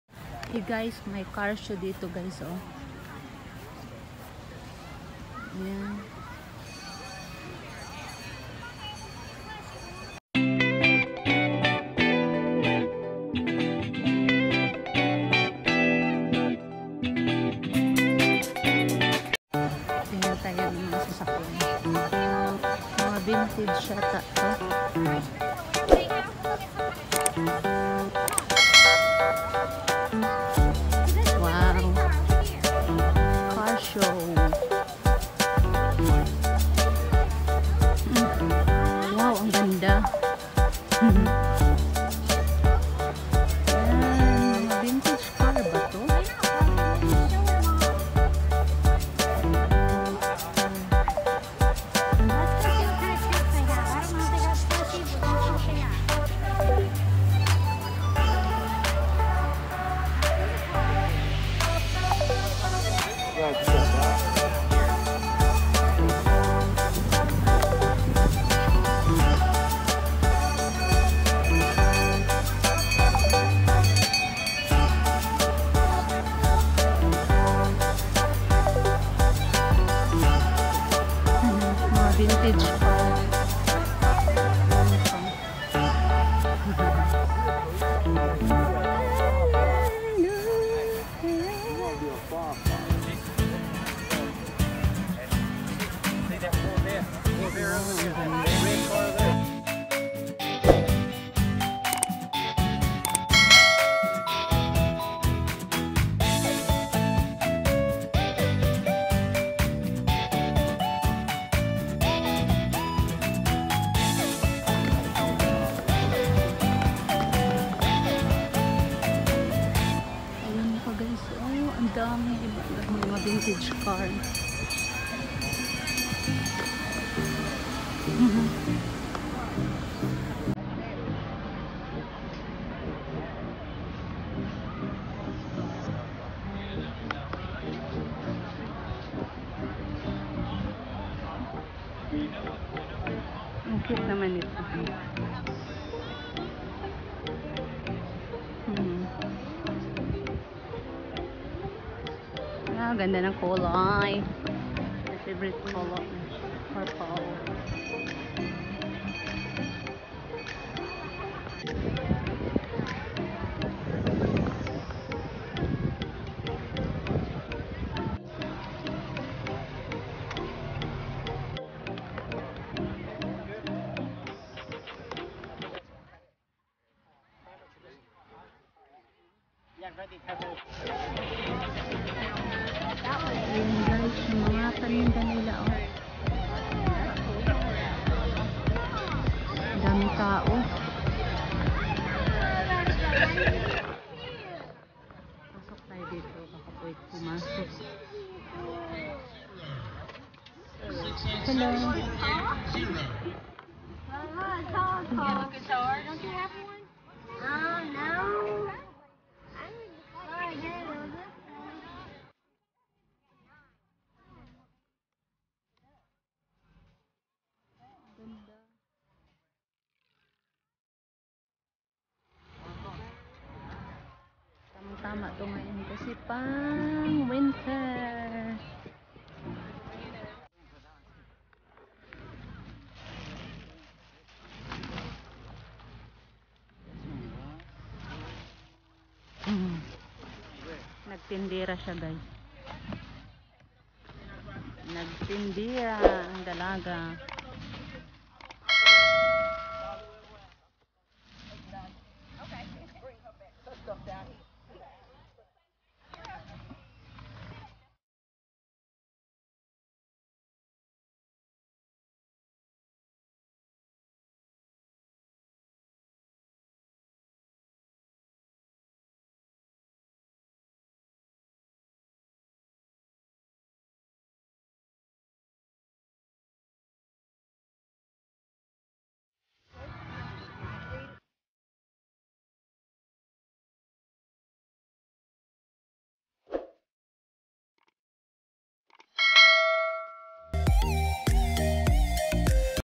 VINTAGE CAR SHOW AT THE PARK | Cathy F Official | FILAM Couple | simpleng kinabuhi sa amerika
At the park we saw different kind of Vintage car